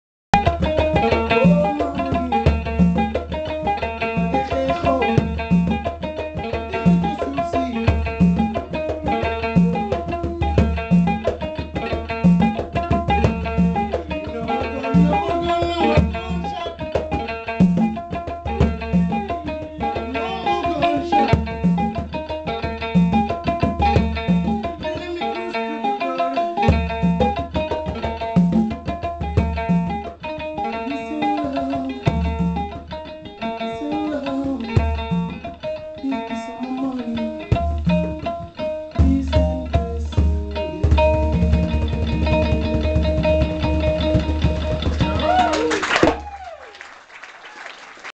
Sana Ndiaye & the Ekonting Peace Band
West African Traditional Music from the Gambia River Delta Region at Brooklyn Folk Festival 2009